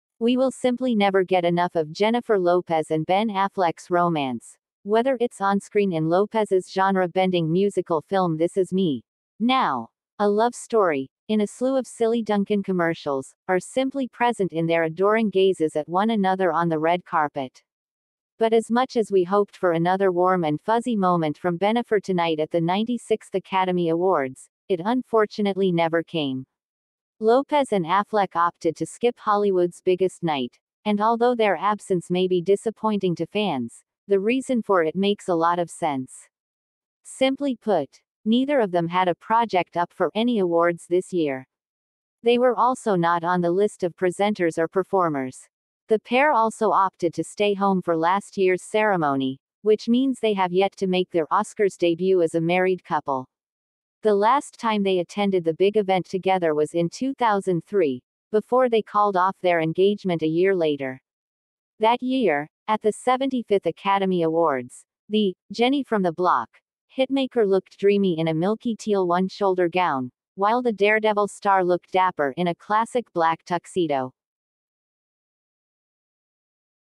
Why Jennifer Lopez and Ben Affleck Skipped the 2024 Oscars
Jennifer Lopez and Ben Affleck chose to skip the 2024 Oscars for personal reasons, which they did not disclose publicly. Their absence sparked speculation among fans and media outlets, but the couple has not provided any specific explanation for their decision.